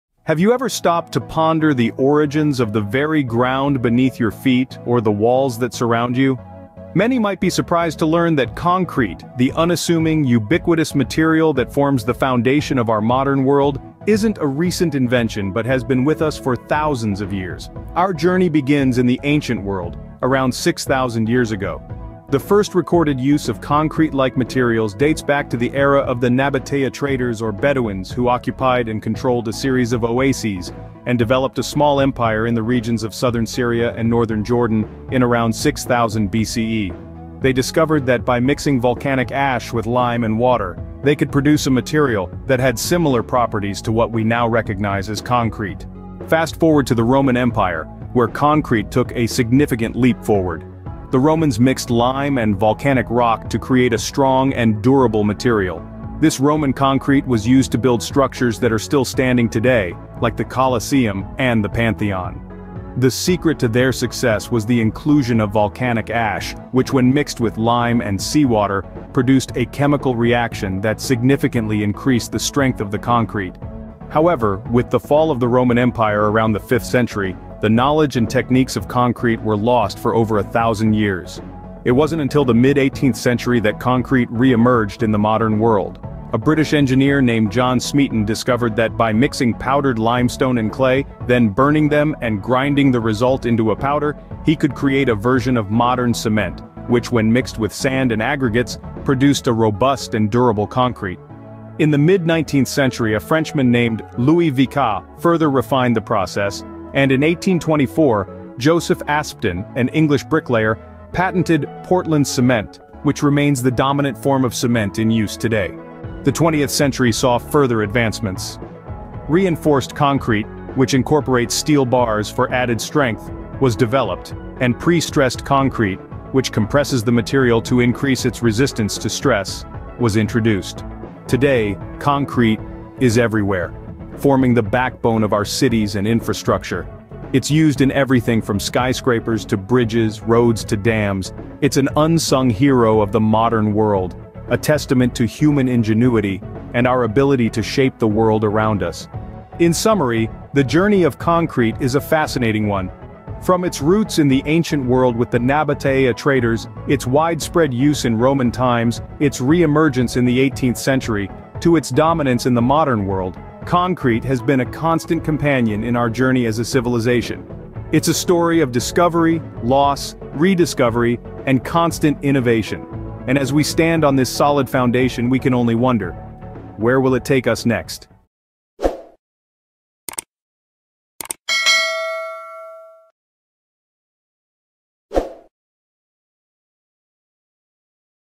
Concrete From Ancient Times to Modern World
Concrete: From Ancient Times to Modern Marvels

00:00:00
The Origins of Concrete

00:00:46
Concrete in the Roman Empire

00:01:13
The Dark Ages and Rediscovery

00:01:55
Advancements in the Twentieth Century

00:02:11
Concrete in the Modern World

00:02:29
The Journey of Concrete